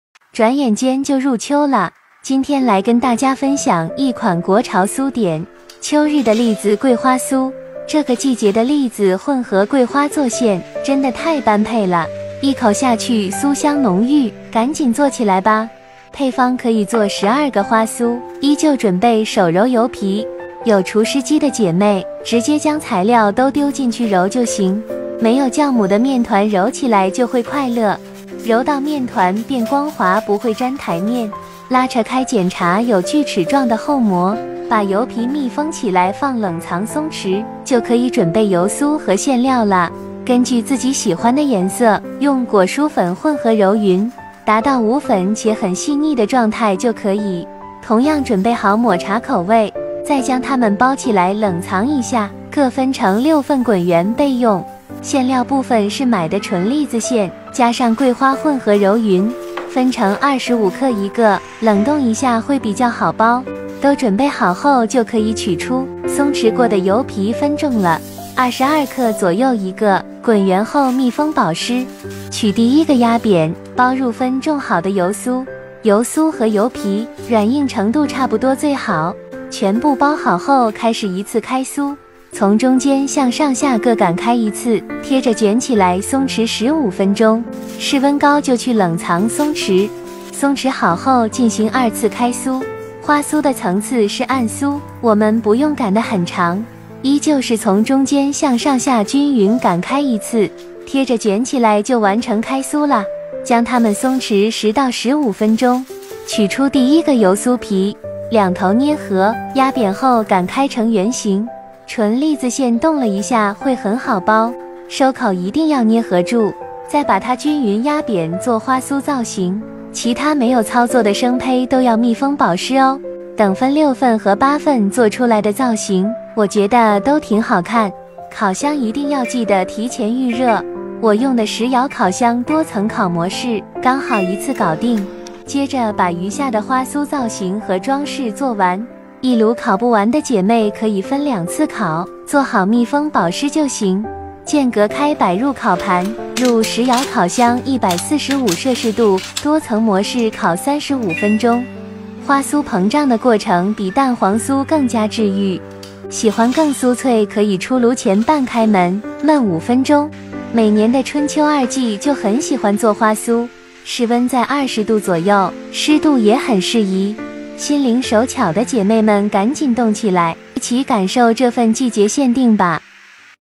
秋日甜點🍂 奶香栗子桂花酥🌰中式甜點教程｜季节限定的奶香栗子和桂花做馅，手工开酥層層酥皮，精緻好看又好吃｜長輩看了都特別喜歡，還很適合做伴手禮🌟
花酥油皮配方：
（可做12个/22g）
中筋麵粉135g
軟化黄油 54g
细砂糖 20g-25g
水 55g

花酥油酥配方：
（可各做6个/13g）
金色
南瓜粉 5g
低筋麵粉 45g
軟化黄油 30g

抹茶
抹茶粉 4g
低筋麵粉 46g
軟化黄油30g

馅料部分：
可做12个/25g
成品栗子馅 300g
桂花 若干

烘烤：
- 石窑烤箱多層烤模式
- 145℃烤30-35分钟，出炉前半开门焖5分钟。

- 酥皮点心最适宜做的室温在20度以下，秋冬天气干燥，油皮、油酥都要随时做好保湿工作。
- 开酥时在台面铺上保鲜膜，擀面杖也包上保鲜膜，这样水油酥皮不容易破。
- 烘烤温度是参考，结合自己烤箱和成品状态调整，出炉前开一点门，焖5分钟，酥皮会更酥一些。